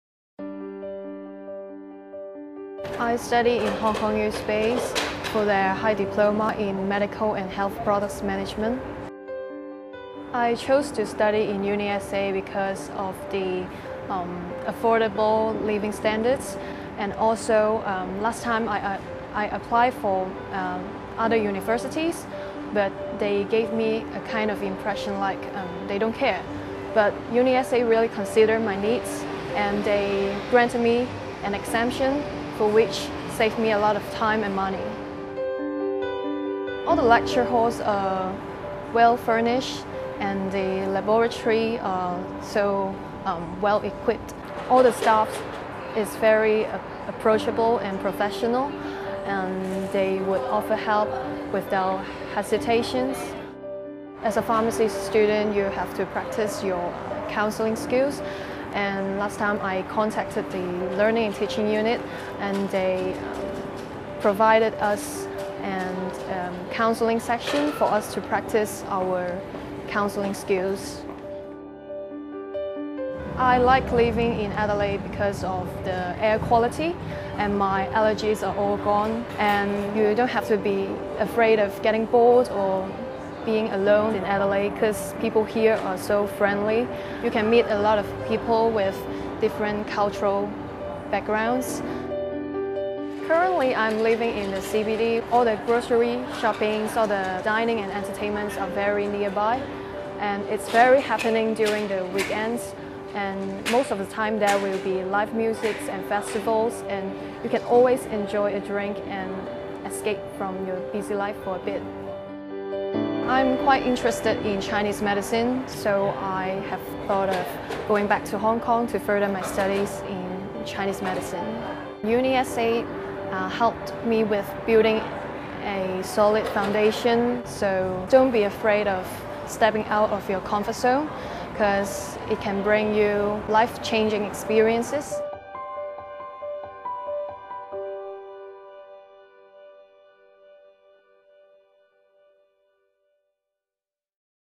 Cheuk Ying Tai (Janice) Bachelor of Pharmacy Testimonial
Hong Kong's Janice speaking about her experience at UniSA studying Pharmacy and how welcome she feels in Adelaide.